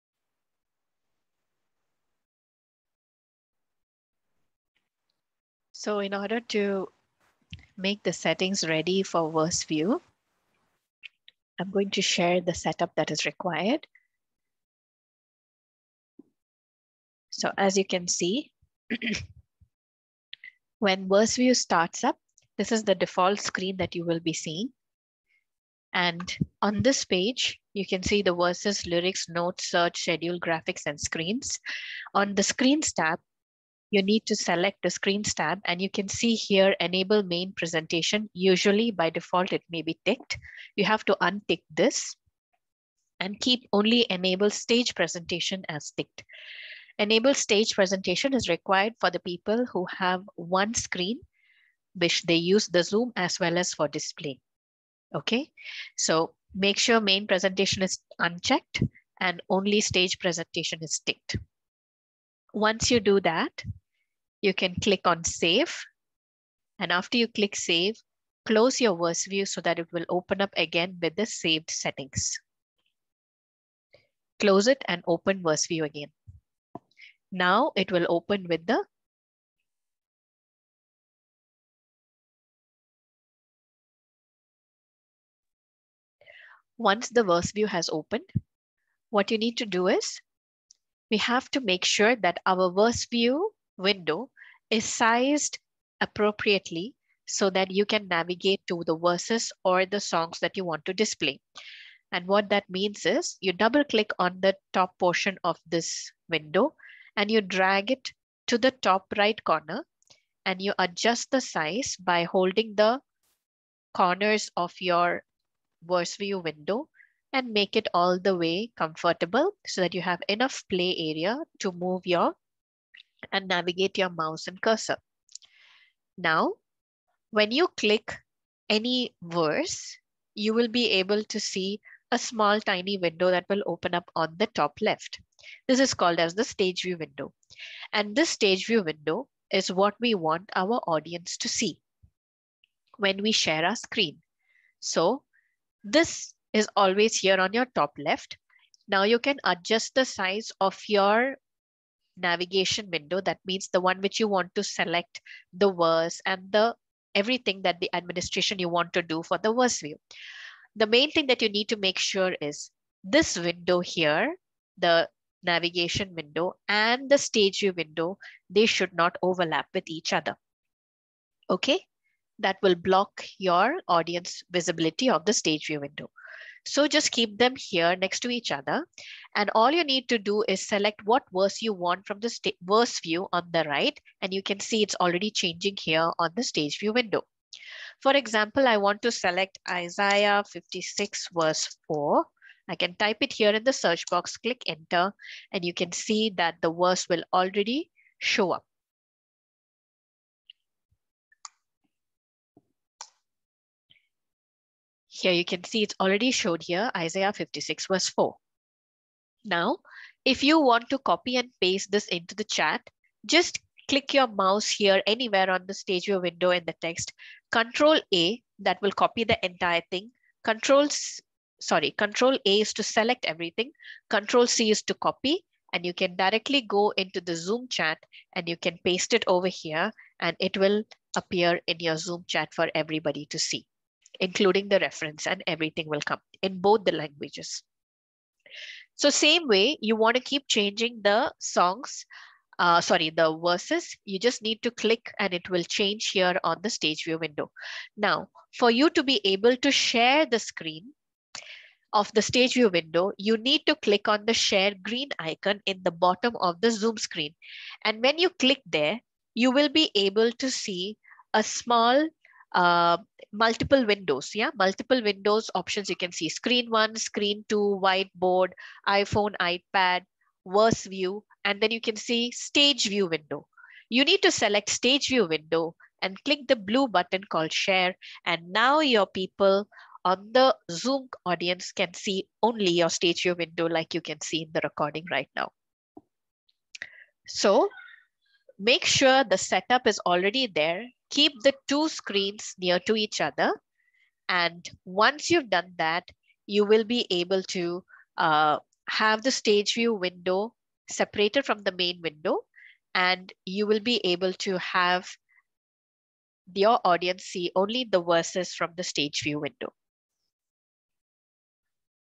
How to setup StageView For Verse View for Zoom Display
This Video explains how to easily display VerseView in Zoom for people who have only 1 Display Screen